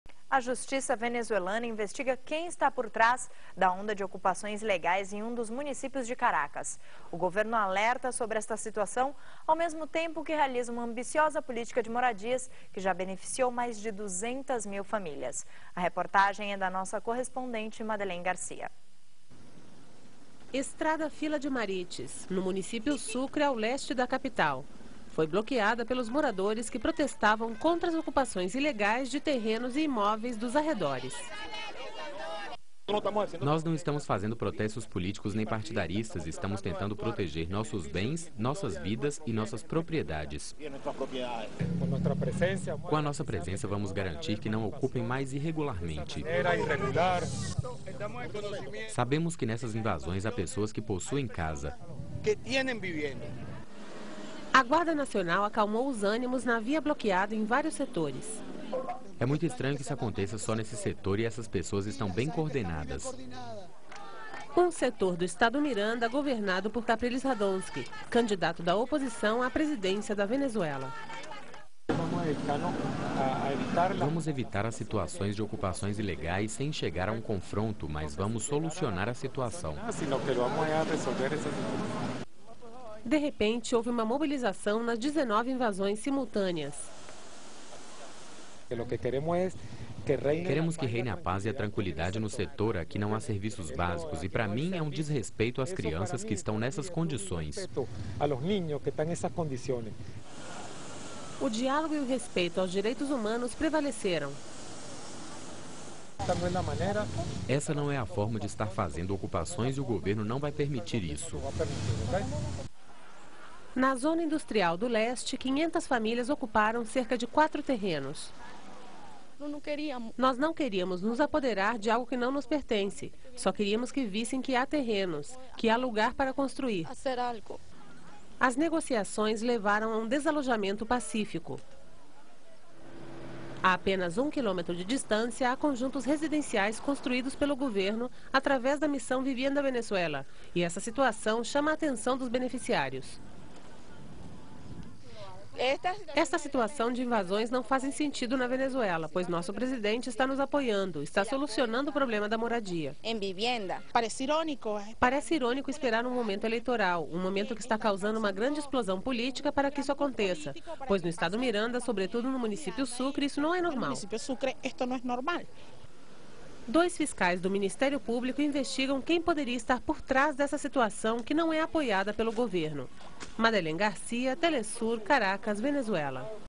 Governo venezuelano atento às ocupações ilegais de terrenos
A polícia venezuelana investiga quem está por trás das ocupações ilegais no município Sucre no estado de Miranda. O governo alerta sobre a situação e ao mesmo tempo elabora um projeto de moradia ambicioso. Moradores bloquearam as vias em protesto contra as ocupações. Foram 19 invasões simultâneas por cerca de 500 famílias em quatro terrenos. teleSUR